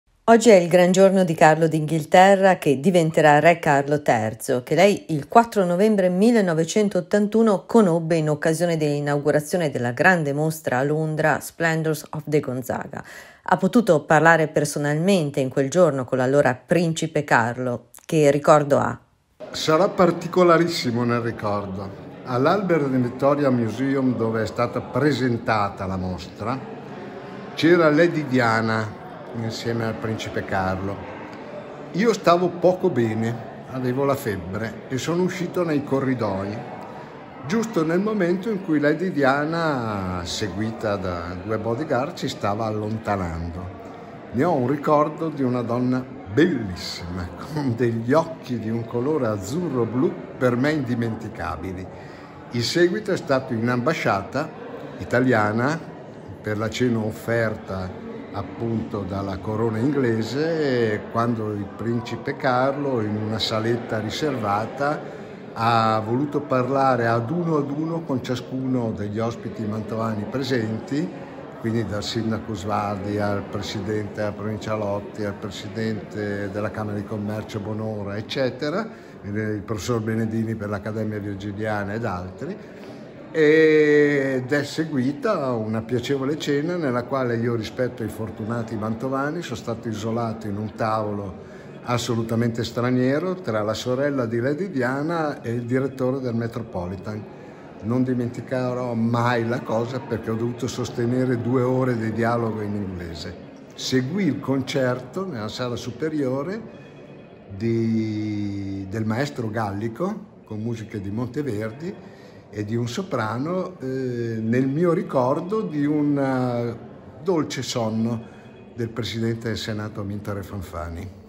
1981, la delegazione della Città di Mantova incontra il futuro re Carlo III
1981,quando  la delegazione della Città di Mantova incontrò il futuro re Carlo III